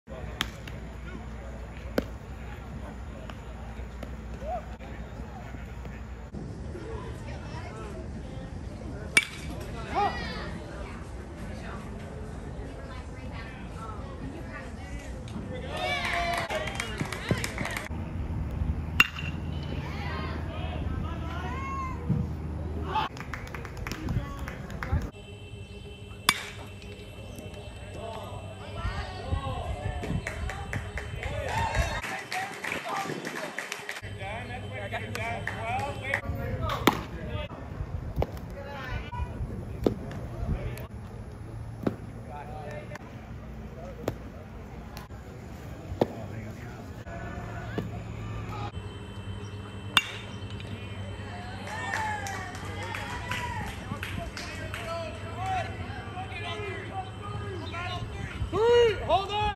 Is Maddox Miesse a top catcher in the '22 class?
Maddox Miesse made a name for himself this weekend at the Five Tool World Series. The RHH hit .400 with three home runs and 8 RBI. Fluid through the zone and creates pop off the bat. Shows some tools behind the dish with an accurate arm and above-average blocking ability. He won't be uncommitted for long.

East Central (TX) 2022 - C
6-0, 175 lbs.